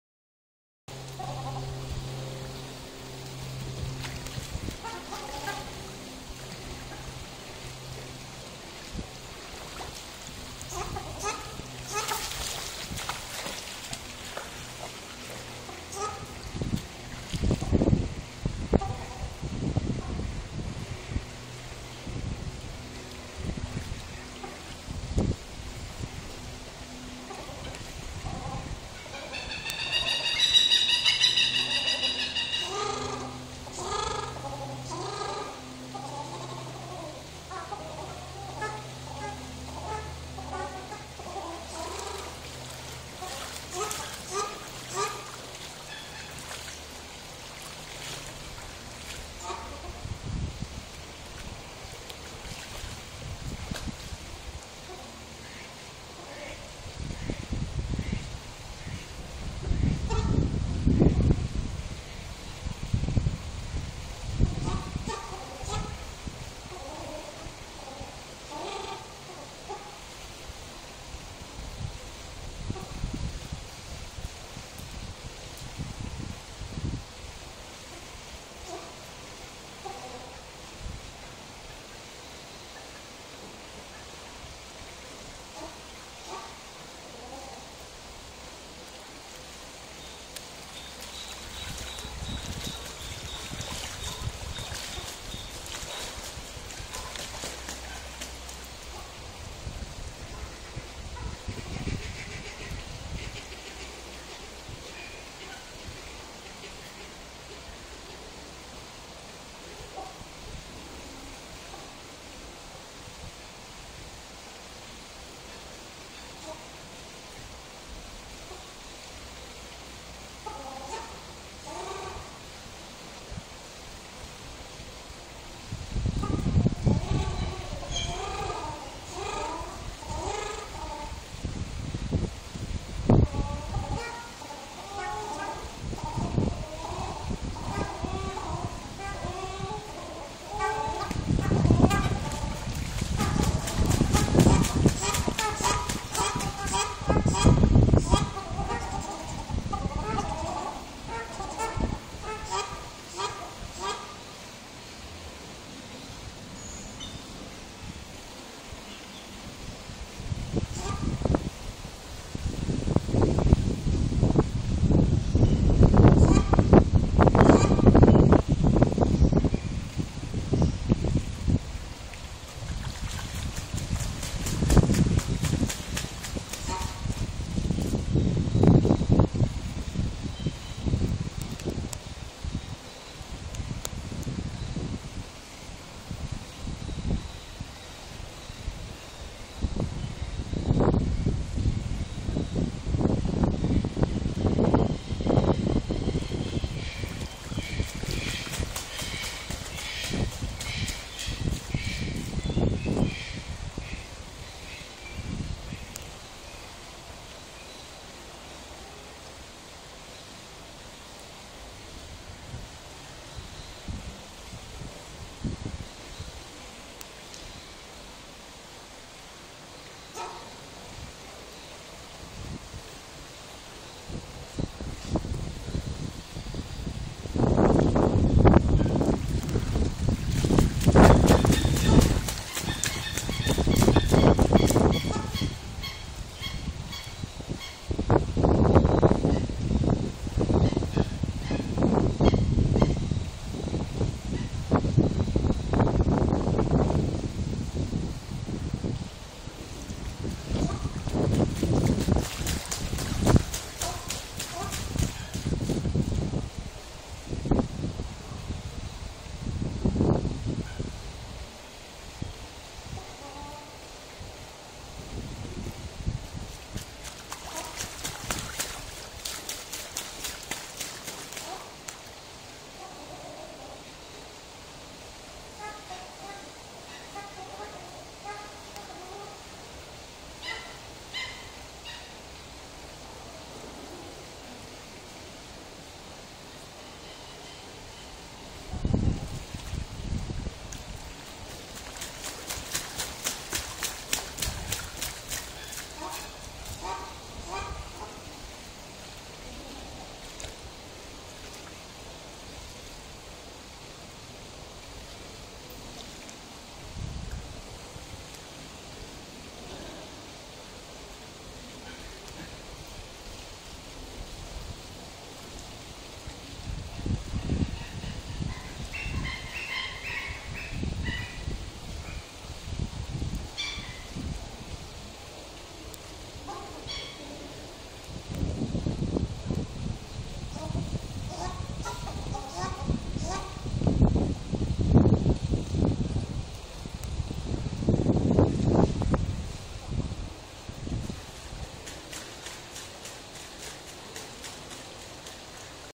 マナヅル「カズオ」の水浴び1
神戸花鳥園のマナヅル「カズオ」の水浴び。